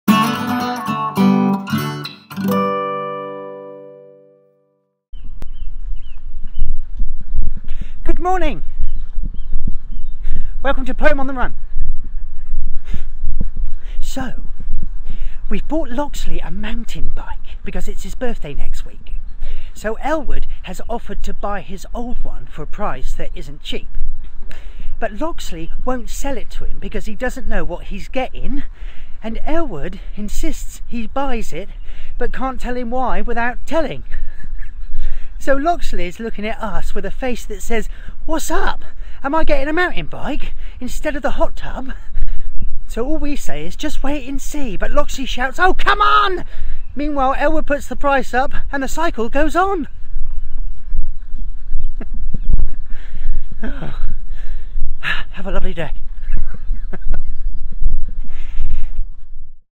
Birthday Bike Poem
BGT 2018 Misheard Lyric Funnyman Norfolk Comedian continues his "Poem-On-The-Run" series throughout lockdown.